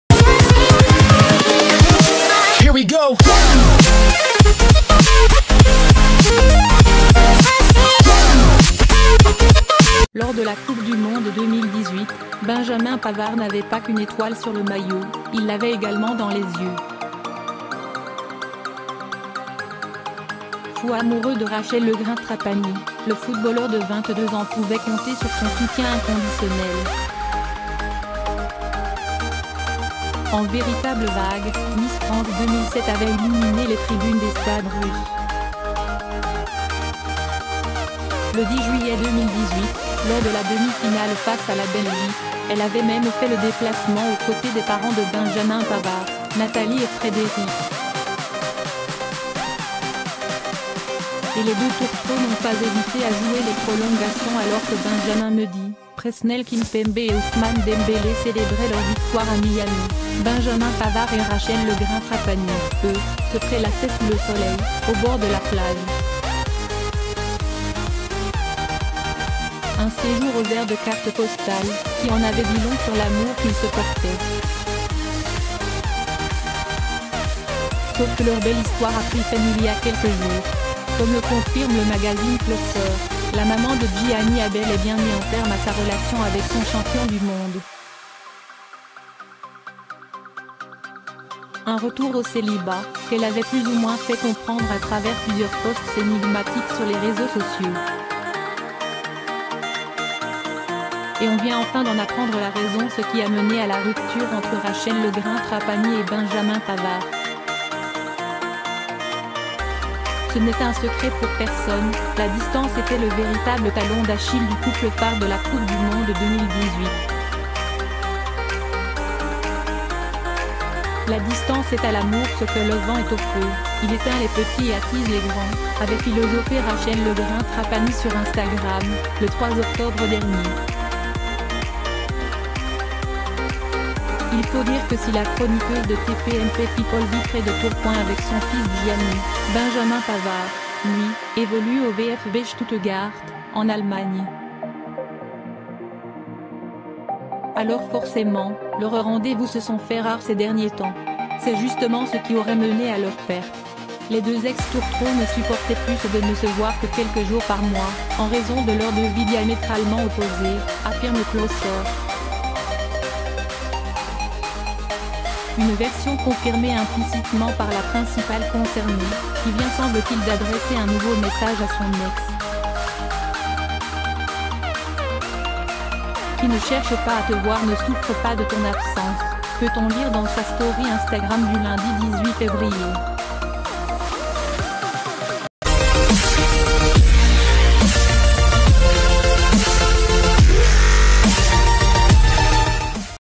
Rachel Legrain-Trapani et Benjamin Pavard : les raisons de leur rupture dévoilées
Lors de la Coupe du monde 2018, Benjamin Pavard n’avait pas qu’une étoile sur le maillot, il l’avait également dans les yeux. Fou amoureux de Rachel Legrain-Trapani, le footballeur de 22 ans pouvait compter sur son soutien inconditionnel. En véritable WAG, Miss France 2007 avait illuminé les tribune...